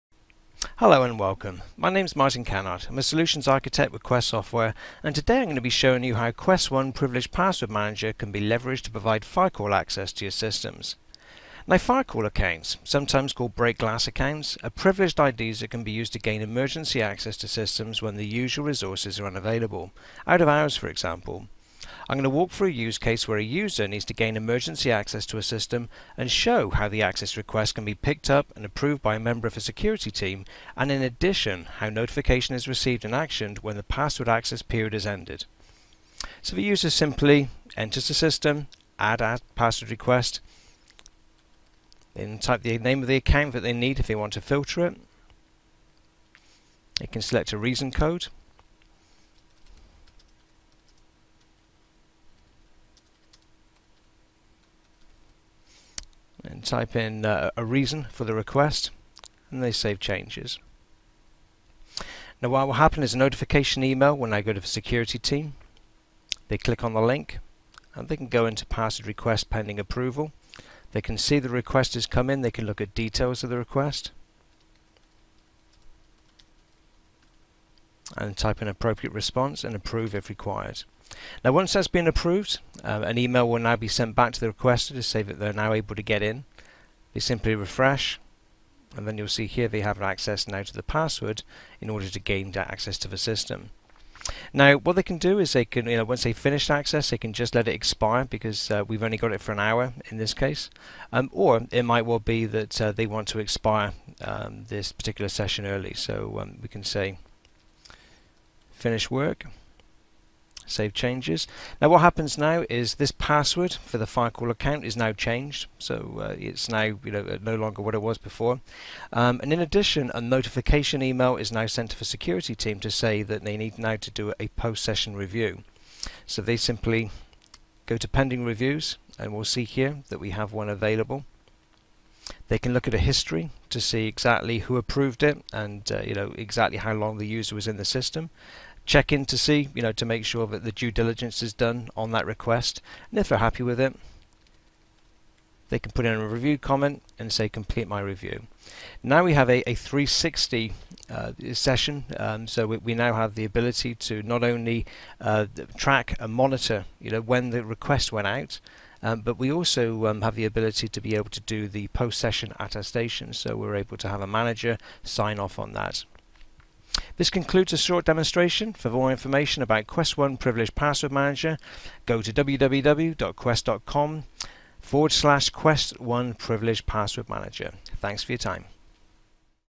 How to grant firecall access using One Identity Privileged Password Manager
Learn how to grant firecall access using Privileged Password Manager, One Identity's solution automating and securing the management of privileged accounts and passwords.